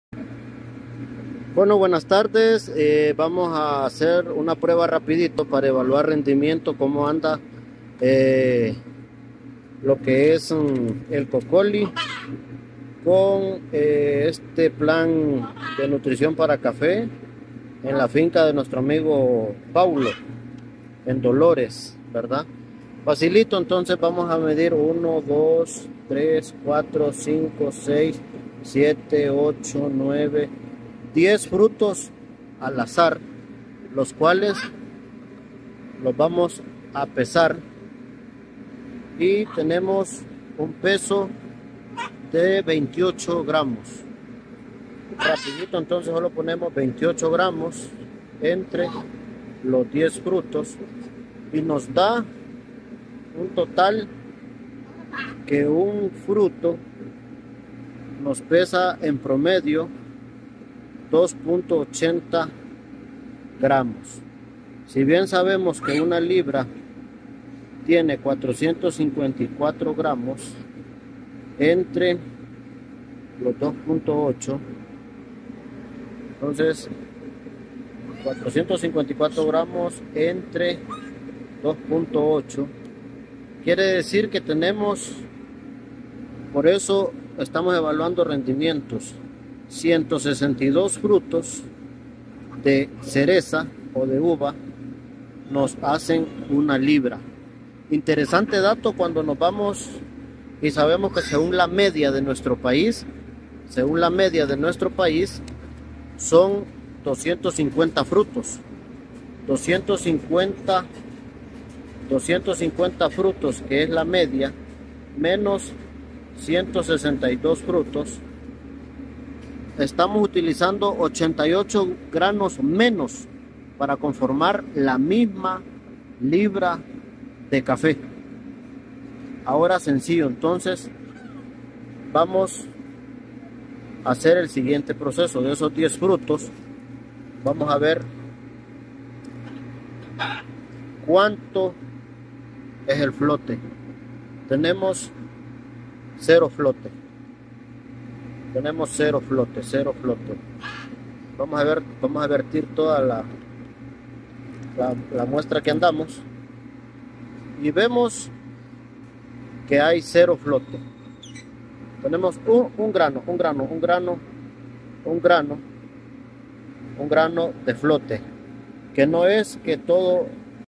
Honduras customer proves that the effect of cocoly fertilizer is very good with experimental data
In this video, this Honduras customer weighed coffee beans with cocoly fertilizers. By calculating, 1 pound can harvest 162 fruits, but using other fertilizers can harvest 250 fruits with the same weight. This proves that after using Cocoly granular water-soluble fertilizer, the weight and volume of coffee fruits will increase, and the yield of farmers will also be increased.

En este video, este cliente de Honduras pesaba granos de café con fertilizantes cocoly. Al calcular, 1 libra puede cosechar 162 frutas, pero el uso de otros fertilizantes puede cosechar 250 frutas con el mismo peso. Esto demuestra que después de usar Cocoly, el peso y los Yiels de las frutas de café aumentarán.

Cocoly is the first granular water-soluble fertilizer in China. Focusing on Cocoly for 11 years, exporting to 46 countries.